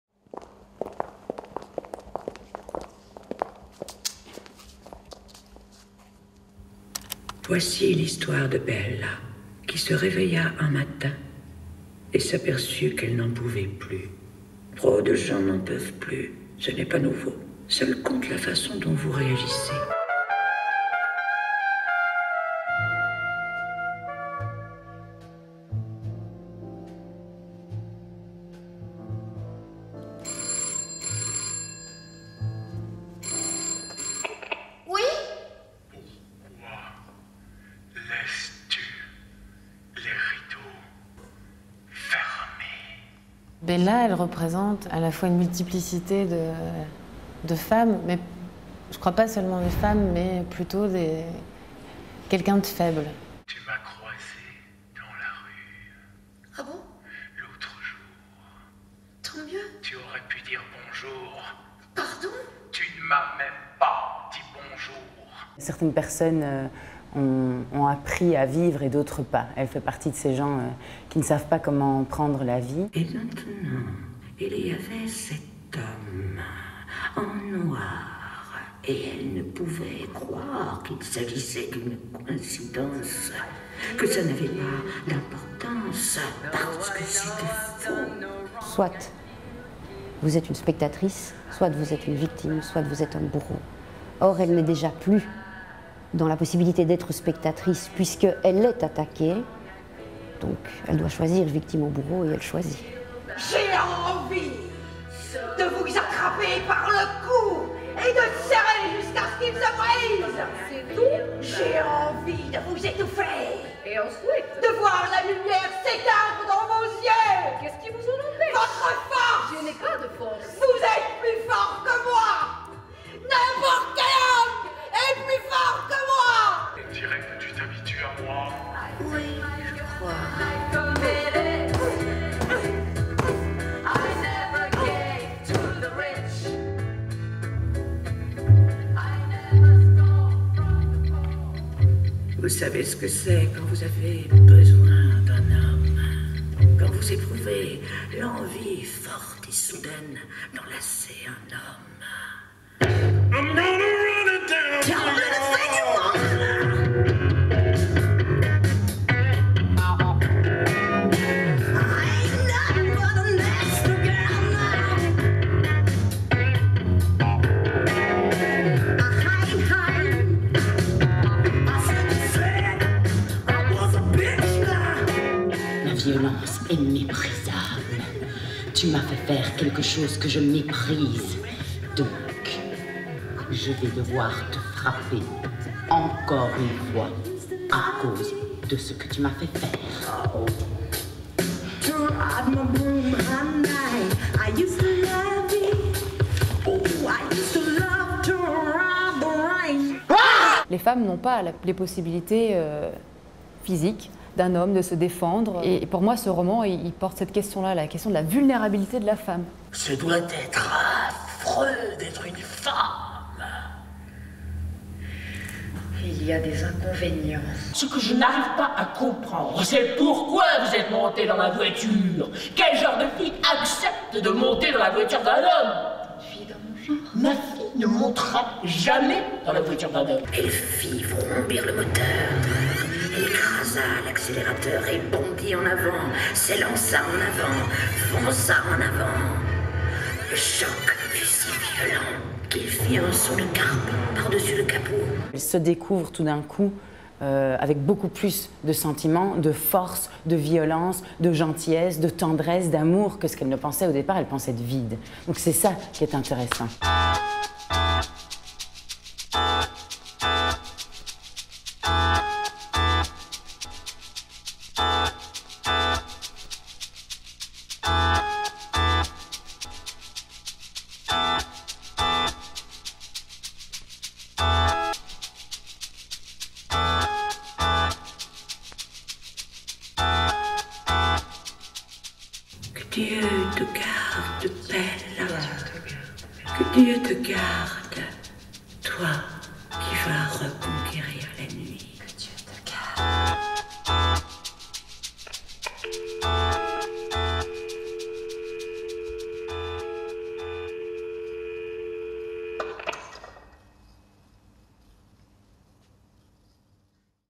Dirty week end d'Helen ZAHAVI / Mise en scène Jacques DELCUVELLERIE
Au Théâtre National du 28.02 au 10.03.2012 -

« Voici l'histoire de Bella qui se réveilla un matin et s'aperçut qu'elle n'en pouvait plus »

Ainsi commence le très étonnant roman noir d'Helen Zahavi Dirty week-end, qui causa un tel choc en Angleterre à sa parution (1991), qu'une intervention au parlement exigea son interdiction. C'était la première oeuvre d'une toute jeune femme, elle avait 25 ans...